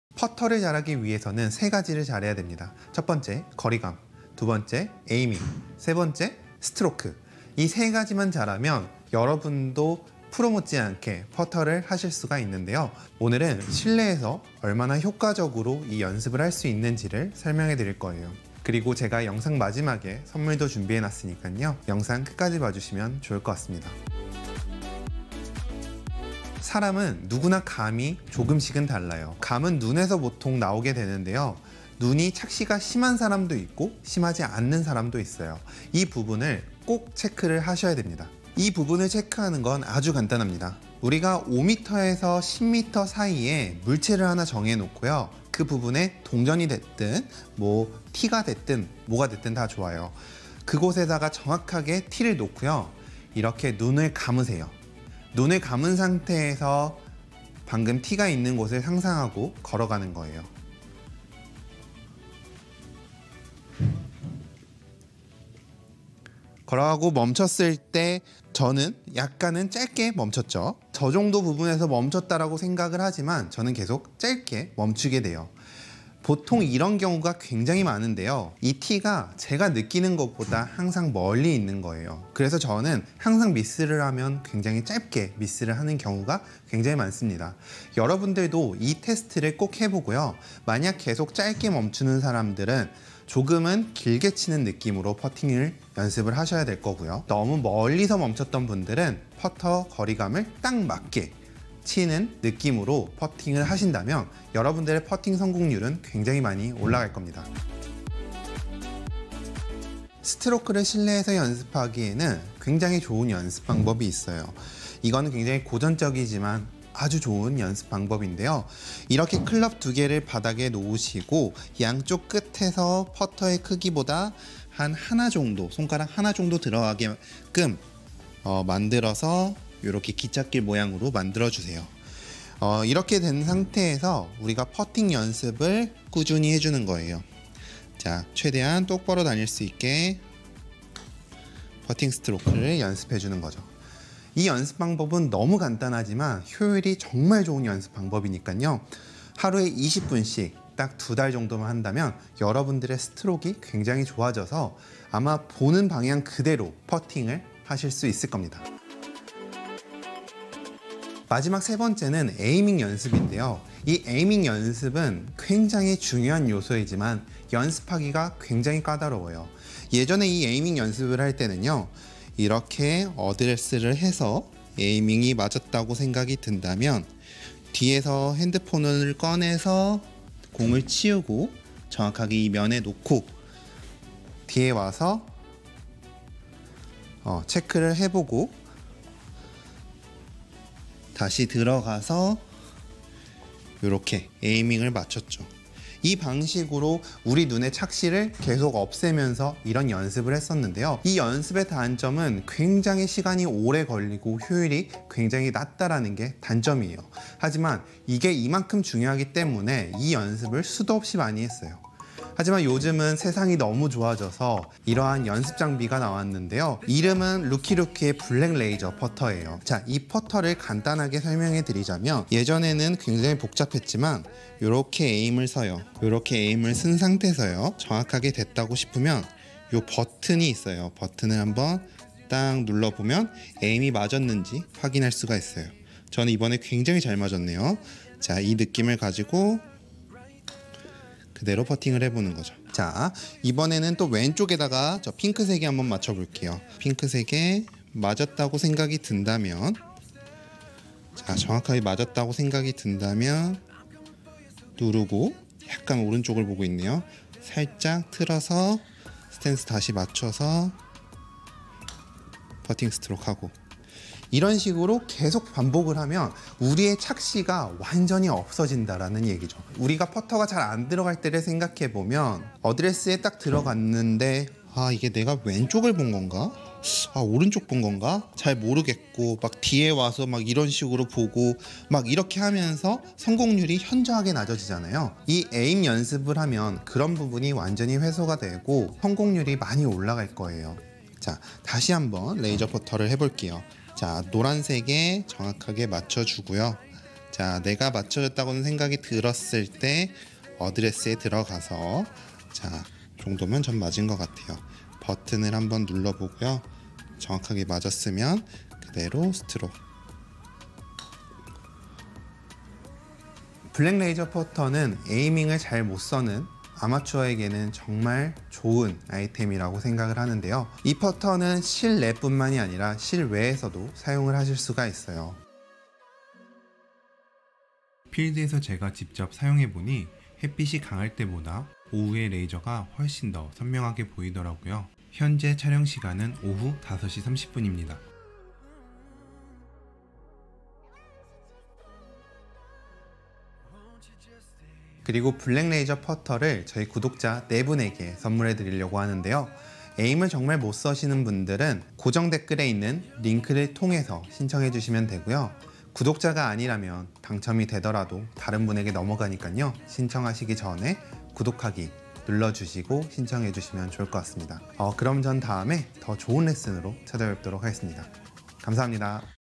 [허석프로]루키루키 블랙 레이저 퍼터 허석프로 리뷰~
ROOKIEROOKIE
허석프로님의 퍼터 레슨~
퍼터를 잘하기 위한 3가지 방법!
1.거리감 2.에이밍 3.스트로크  이 3가지를 잡자~!
실내에서 효과적으로 3가지 연습을 어떻게 할수 있는지~ 확인해 보세요^^

레이저퍼터를 가지고 효율적으로 연습해 보세요^^